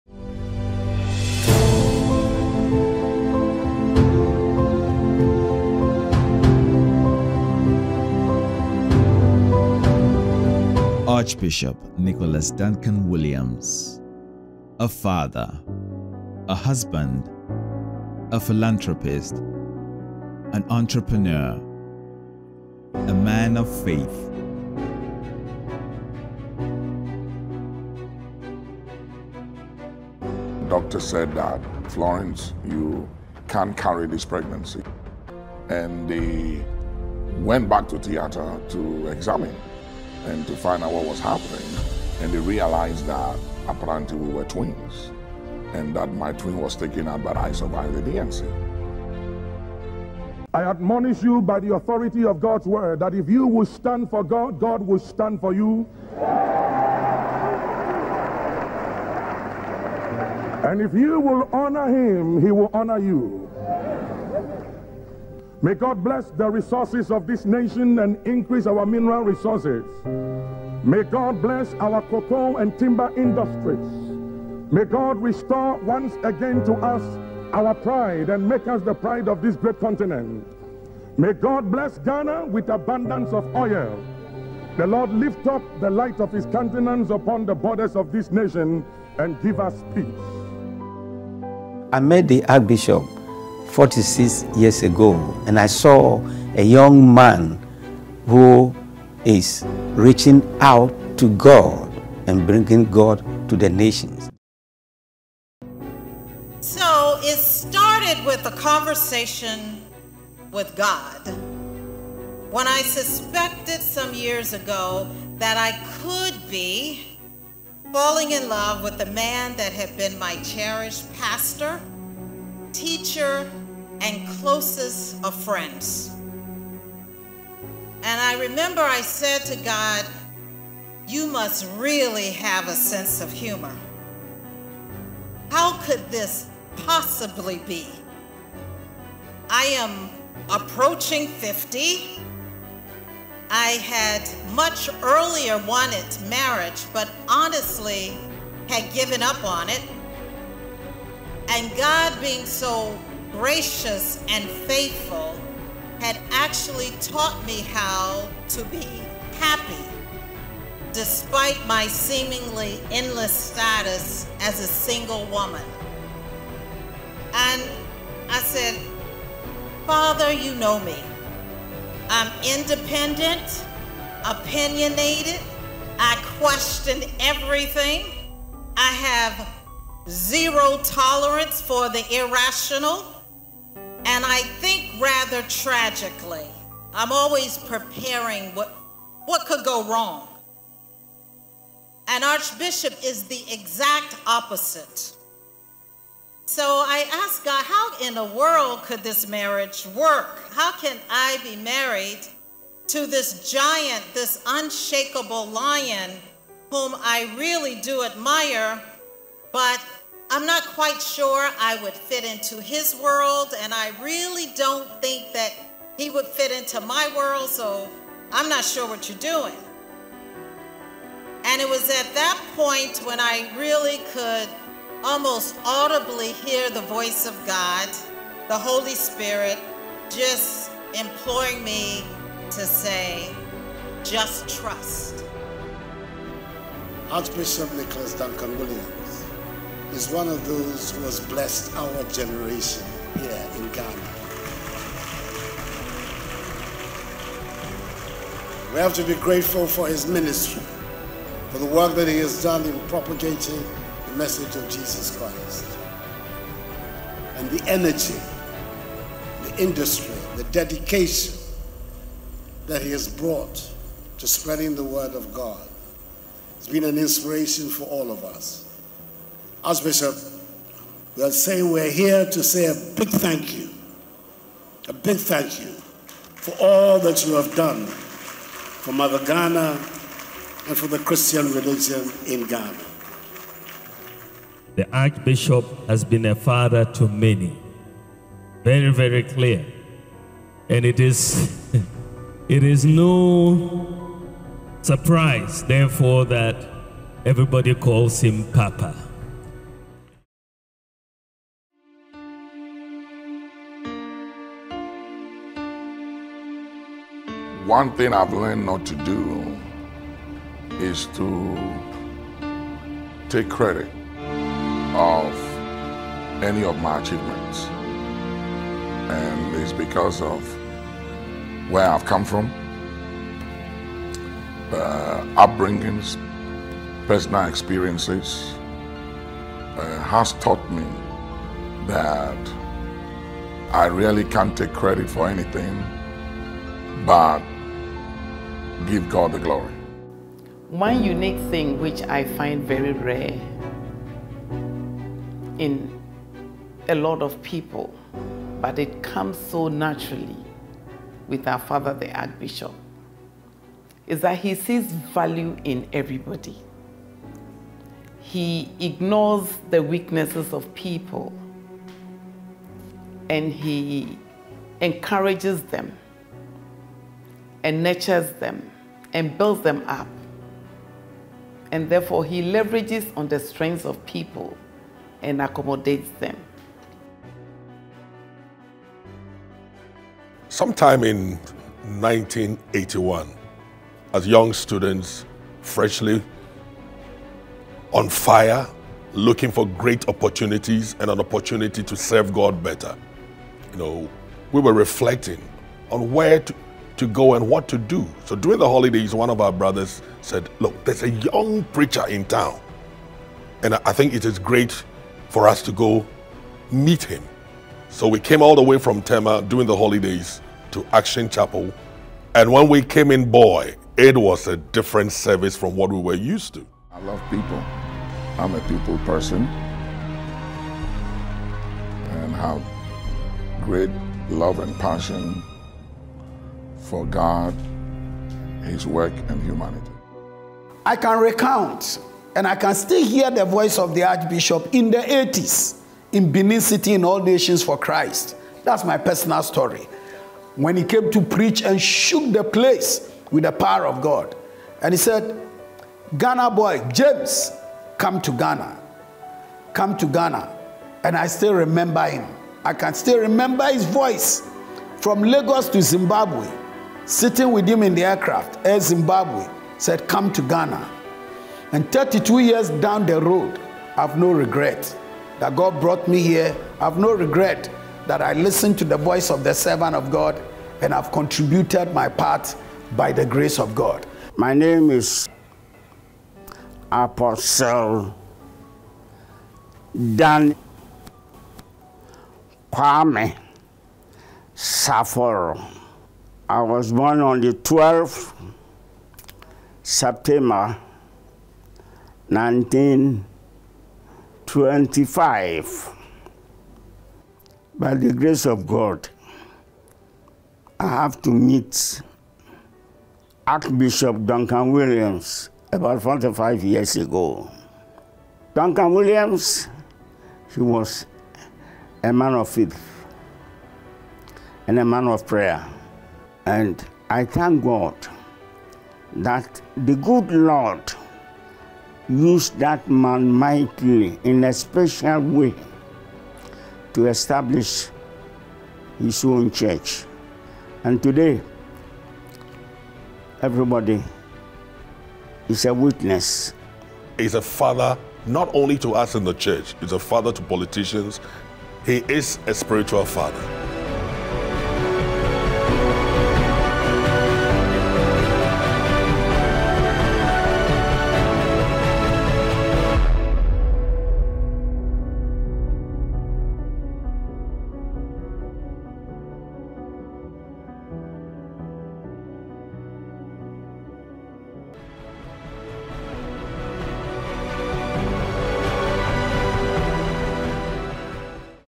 Honoring The Visionary And Celebrating The Vision: Archbishop Nicholas Duncan Williams | Part 1
Journey with us as we honor the visionary and celebrate the vision; taking it back to where it all began. Archbishop Nicholas Duncan-Williams' life story takes us through the impact he has had, as narrated by the many who have witnessed the realization of God's promise in his life.

To stay inspired and empowered in life as a Christian, join our Telegram Channel to receive updates on the best content aligned with Christian values